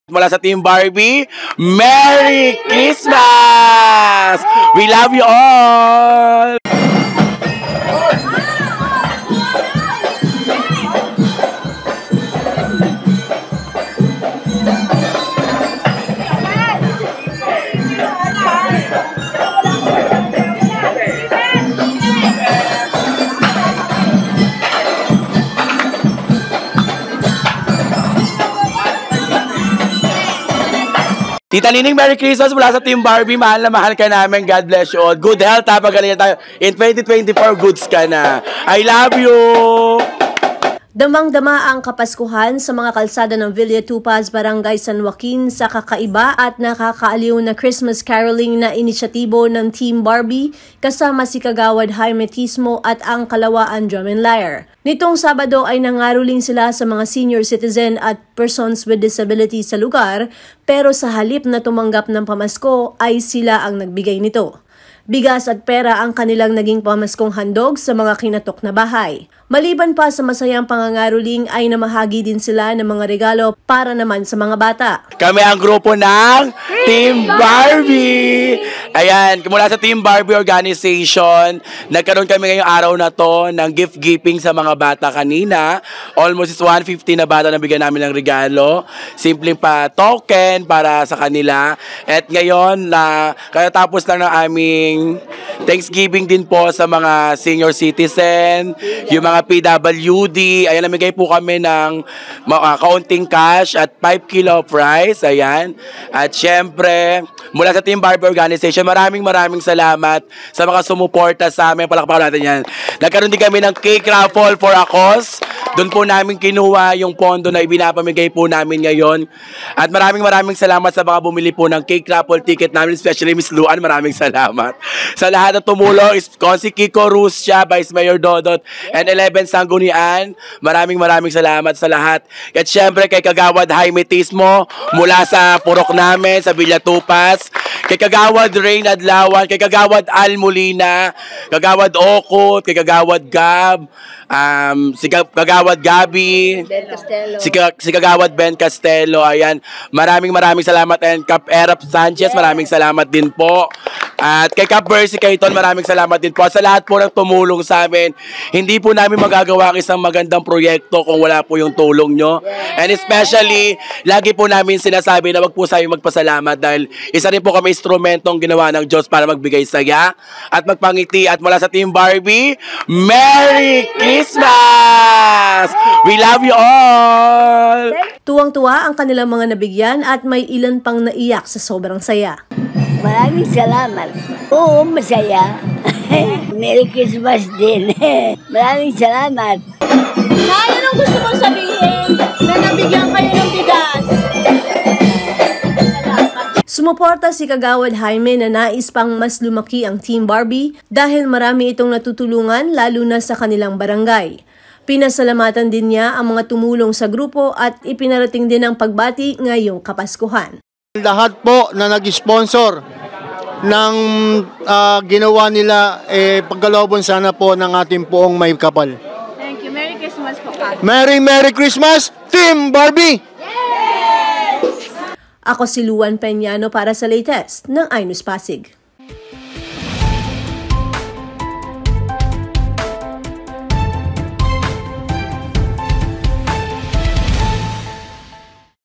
Nakakaaliw, kakaibang Caroling, sinurpresa ang mga senior at person with disabilities | iNews Pasig
Nakakaaliw at kakaibang Caroling, umikot at sinurpresa ang mga senior at persons with disabilities